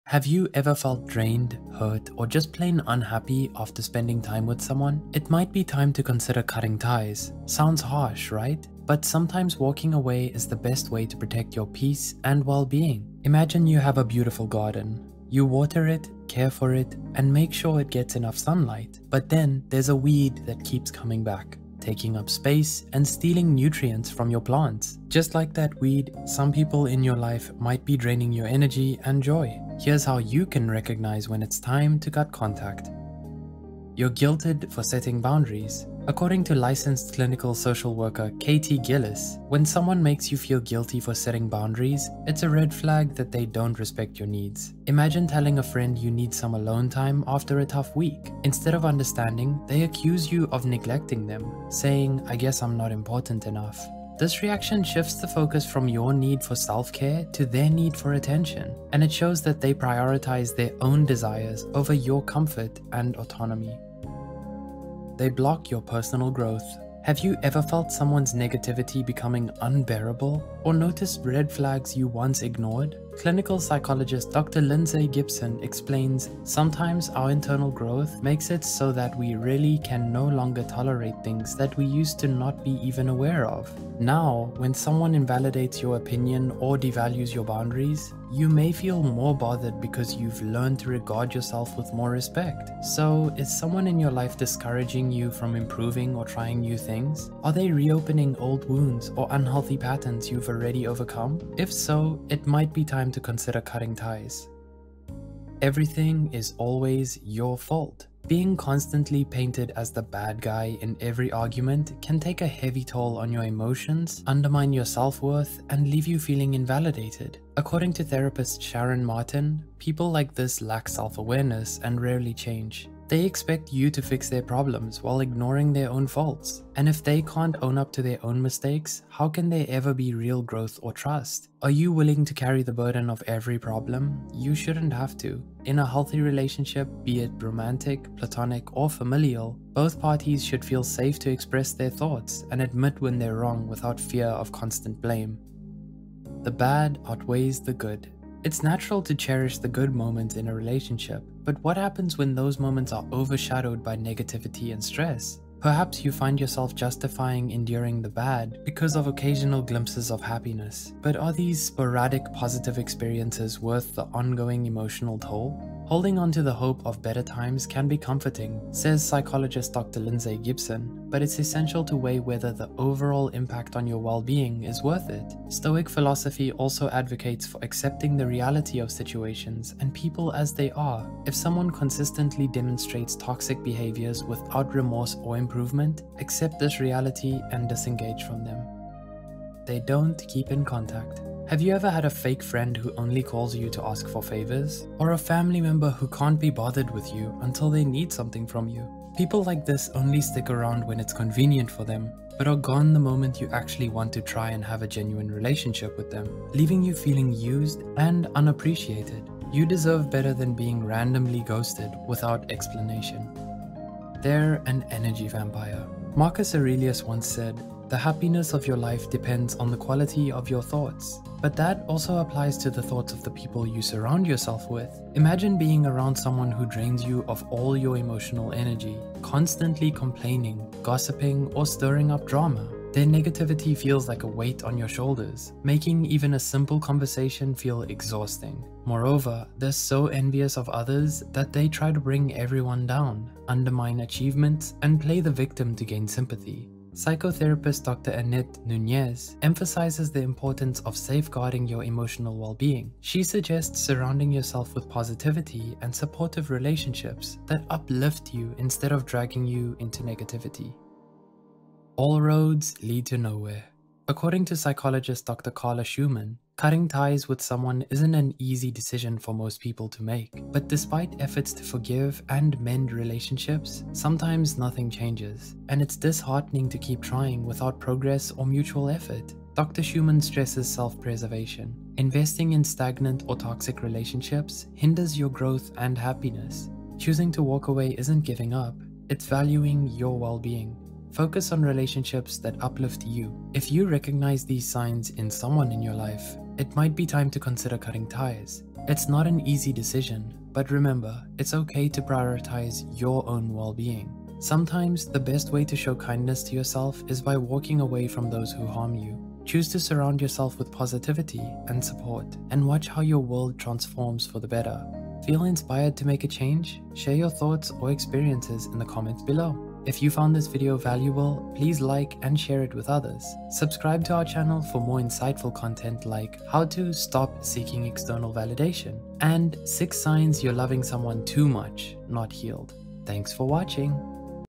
7 Signs You Should Cut All Contact with Someone
Are you wondering if it’s time to cut ties with someone in your life? In this video, we’ll explore the signs you should cut contact with someone and help you recognize when a relationship is doing more harm than good. Whether you’re dealing with toxic friendships, toxic friends, or just feeling drained by someone’s presence, it’s important to know how to protect your well-being.

We’ll cover the warning signs of a toxic relationship, how to spot fake friends, and provide practical advice on how to end a friendship with toxic people. If you’ve been questioning whether someone in your life is a bad friend or if you’re in a toxic friendship, this video is for you. Don’t let toxic friends drag you down—learn how to set boundaries and reclaim your peace.

Writer: Chloe Avenasa
Script Editor & Manager: Kelly Soong
Voice Over: Brandon
Animator: Krisha Que
YouTube Manager: Cindy Cheong